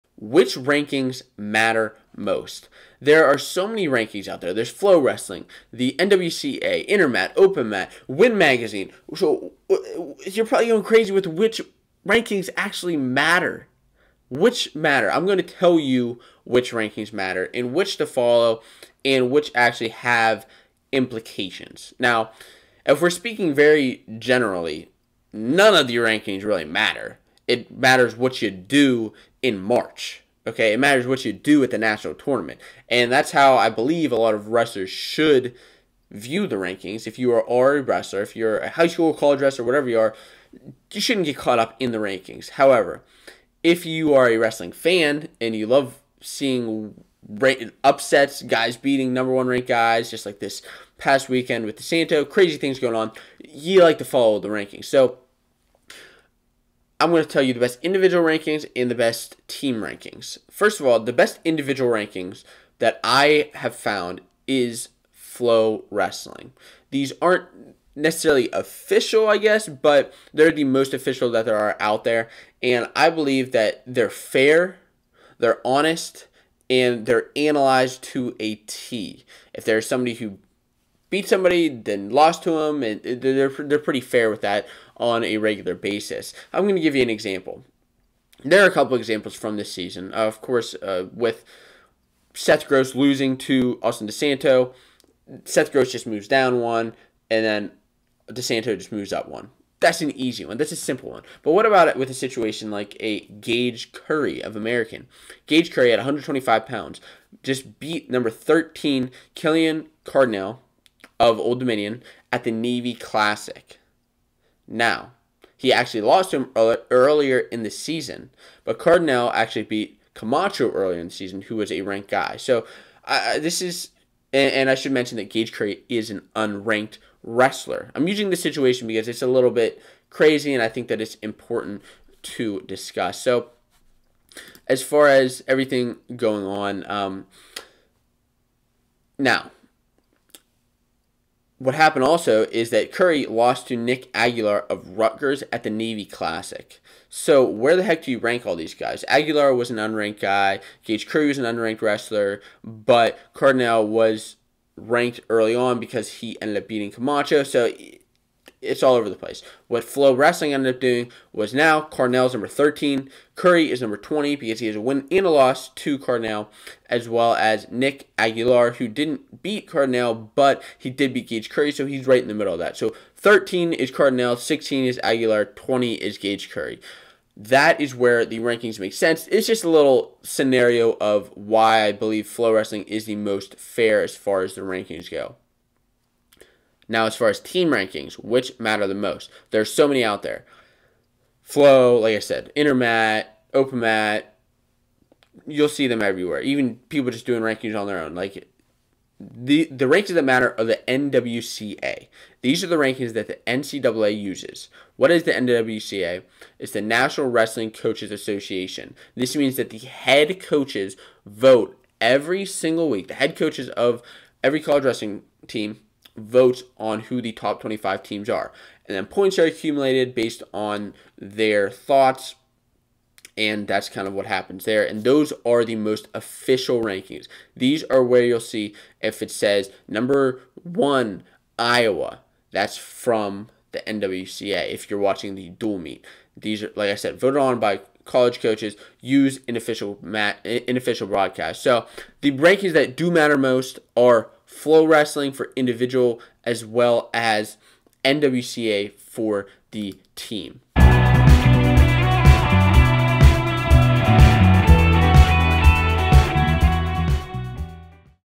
Which Rankings Actually Matter in College Wrestling?
There’s NWCA, FloWrestling, Intermat, The Open Mat, and so many other rankings on the internet. So, which ones really matter?

► 2. WEAR Fanco Wrestling Tees
► 3. FOLLOW Fanco Wrestling:
This week’s stories:

*All graphics are my own, wrestling photos and videos are owned by the NCAA or respective sources.